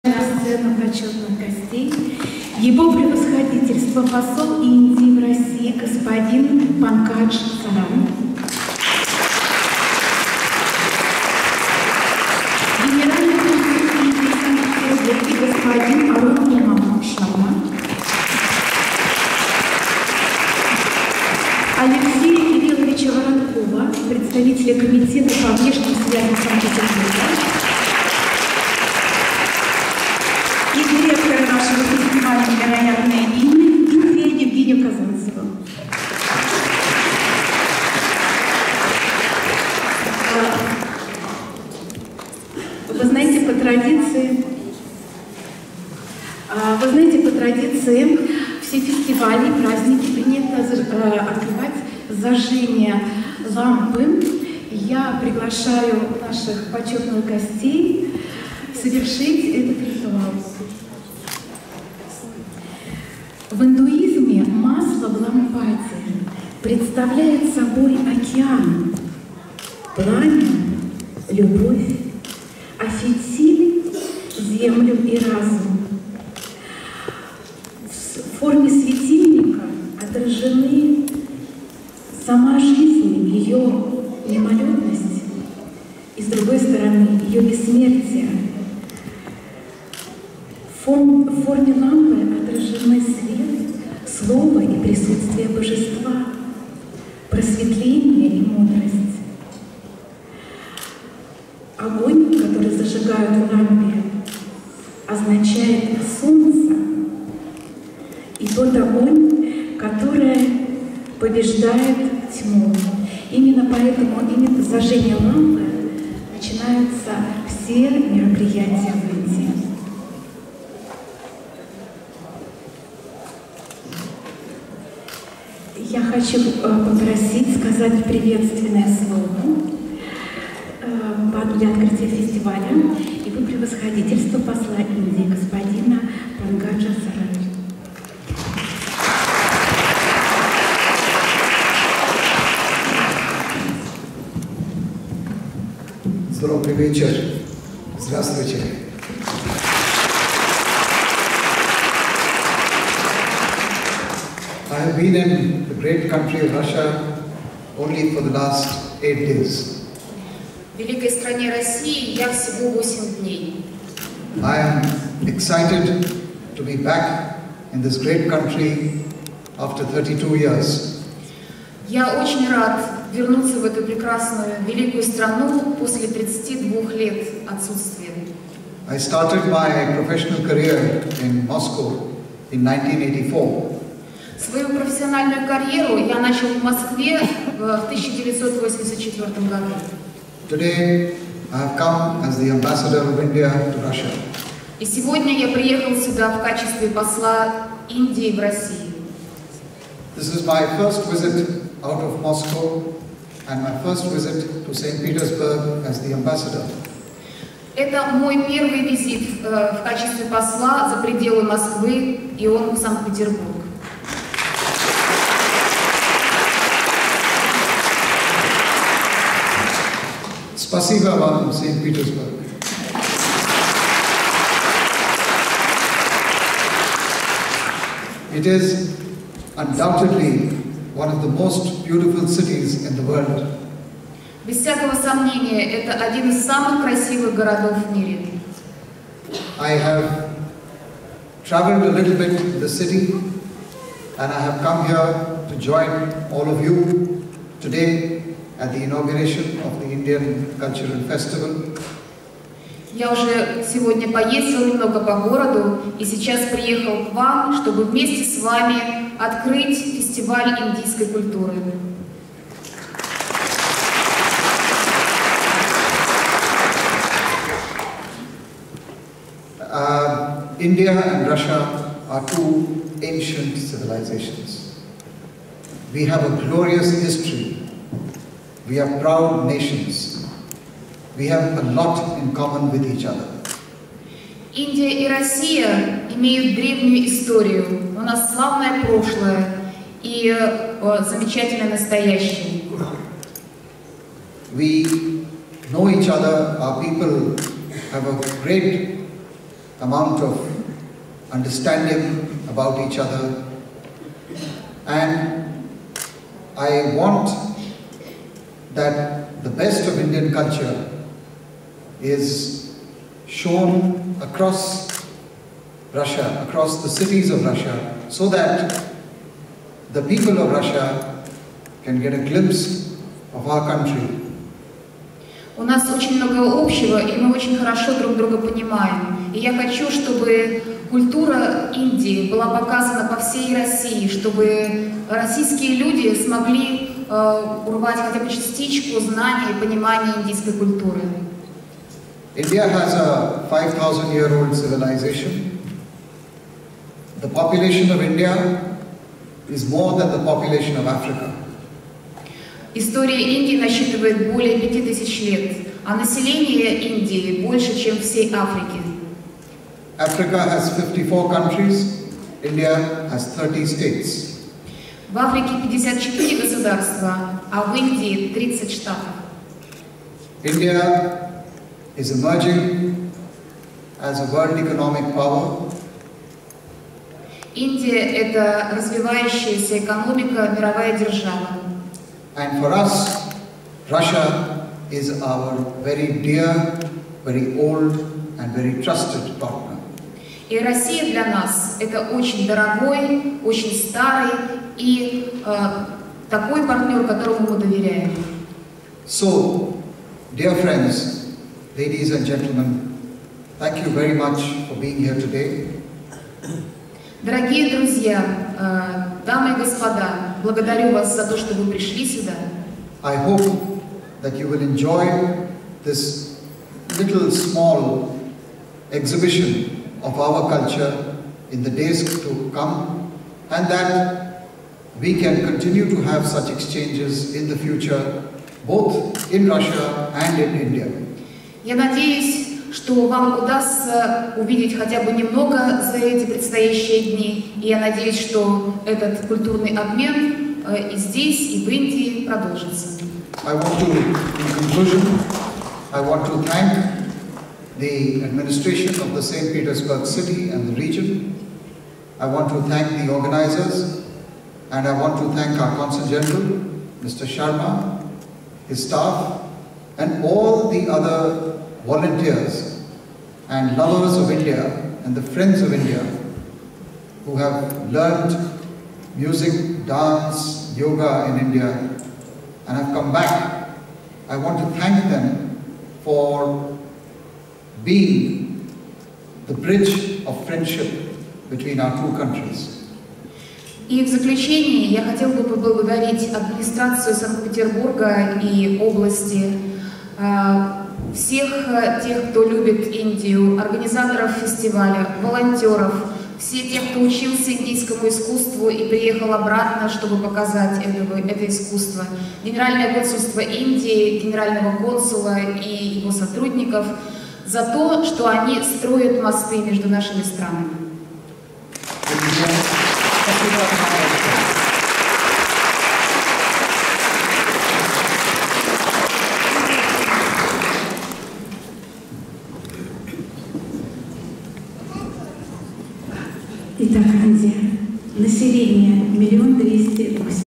Открытие фестиваля "НЕВЕРОЯТНАЯ ИНДИЯ" посол Индии Г-н Панкадж Саран, Алексей Кириллович Воронков
Гадаю на кофе, гадаю на картах, гадаю по рукам (хиромантия и др.). Консультация, встреча – запись по тел: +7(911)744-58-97.
Стаж гадания - с 1986 года.